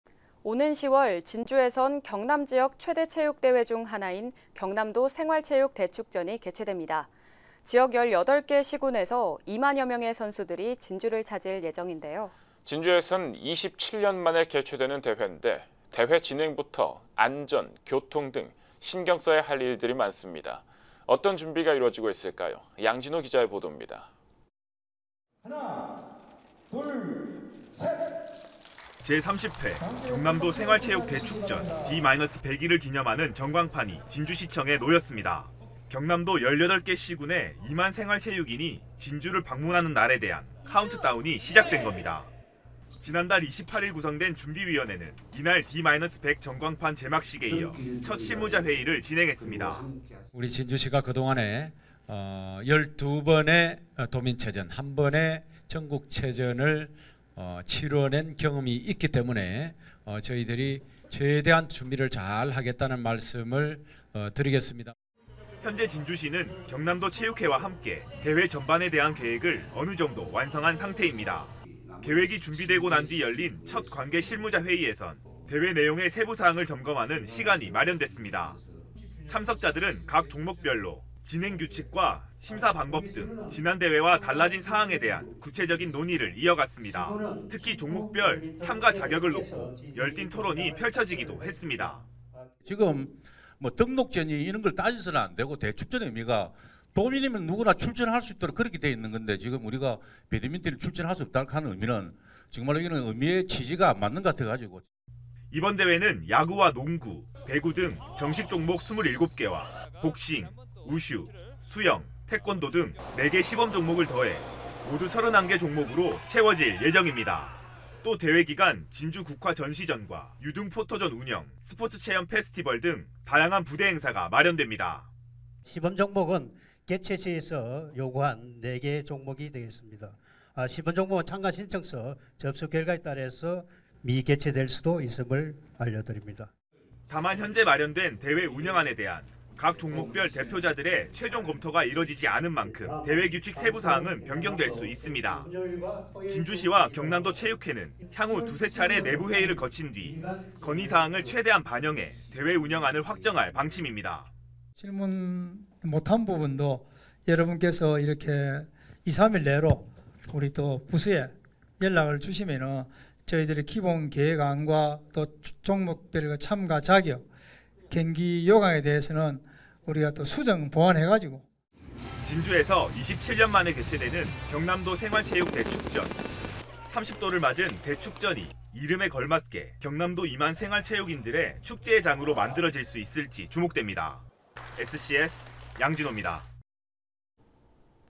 (R) 2만 생활체육인 진주 찾는다..경남도생활체육대전 D-100
【 앵커멘트 】
(여) 오는 10월, 진주에선 경남지역 최대 체육 대회 중 하나인 경남도생활체육대축전이 개최됩니다. 지역 18개 시·군에서 2만여 명의 선수들이 진주를 찾을 예정인데요.
(여) 진주에선 27년만에 개최되는 대회인데, 대회 진행부터 안전, 교통 등 신경 써야 할 일들이 많습니다. 어떤 준비가 이뤄지고 있을까요? 보도에 양진오 기자입니다.

【 기자 】
제30회 경남도생활체육대축전
D-100일을 기념하는 전광판이
진주시청에 놓였습니다.

경남도 18개 시·군의
2만 생활체육인이
진주를 방문하는 날에 대한
카운트다운이 시작된 겁니다.

지난 달 28일 구성된 준비위원회는
이날 D-100 전광판 제막식에 이어
첫 실무자 회의를 진행했습니다.

▶ 인터뷰 : 조규일 / 진주시장
- "우리 진주시가 그동안 열두 번의 도민체전, 한 번의 전국체전을 치러낸 경험이 있기 때문에 저희가 최대한 준비를 잘 하겠다는"
- "말씀을 드리겠습니다. "

현재 진주시는 경남도체육회와 함께
대회 전반에 대한 계획을
어느 정도 완성한 상태입니다.

계획이 준비되고 난 뒤 열린
첫 관계 실무자 회의에선
대회 내용의 세부사항을 점검하는
시간이 마련됐습니다.

참석자들은 각 종목별로
진행 규칙과 심사 방법 등
지난 대회와 달라진 사항에 대한
구체적인 논의를 이어갔습니다.

특히 종목별 참가 자격을 놓고
열띤 토론이 펼쳐지기도 했습니다.

▶ 인터뷰 : 이이형 / 창원시체육회 사업국장
- "지금 등록제니 이런 것을 따져서는 안 되고 대축전의 의미가 도민이면 누구나 출전할 수 있도록 그렇게 돼 있는데, 지금"
- "(창원시) 배드민턴이 출전할 수 없다는 것은 (대축전의) 의미와 취지에 안 맞는 것 같아서..."

이번 대회는 야구와 농구, 배구 등
정식종목 27개와 복싱, 우슈, 수영, 태권도 등
4개 시범종목을 더해 모두 31개 종목으로
채워질 예정입니다.

또 대회 기간 진주 국화 전시전과
유등 포토존 운영, 스포츠체험 페스티벌 등
다양한 부대 행사가 마련됩니다.

▶ 인터뷰 : 서종락 / 경남도체육회 생활체육부장
- "시범종목은 개최시에서 요구한 4개 종목이 되겠습니다. 심범종목은 참가 신청서 접수 결과에 따라 미개최 될 수도 있음을"

다만 현재 마련된 대회 운영안에 대한
각 종목별 대표자들의
최종 검토가 이뤄지지 않은 만큼,
대회 규칙 세부사항은
변경될 수 있습니다.

진주시와 경상남도체육회는
향후 두세 차례 내부 회의를 거친 뒤
건의사항을 최대한 반영해
대회 운영안을 확정할 방침입니다.

▶ 인터뷰 : 지현철 / 경남도체육회 사무국장
- "질문을 하지 못한 부분도 여러분께서 2~3일 내로 부서에 연락을 주시면 저희가 기본 계획안과 종목별 참가 자격,"
- "경기 요강에 대해서는 수정·보완해서... "

진주에서 27년만에 개최되는
경남도생활체육대축전.

30돌을 맞은 대축전이 이름에 걸맞게
경남도 2만 생활체육인들의
축제의 장으로 만들어질 수 있을지
주목됩니다. SCS 양진오입니다.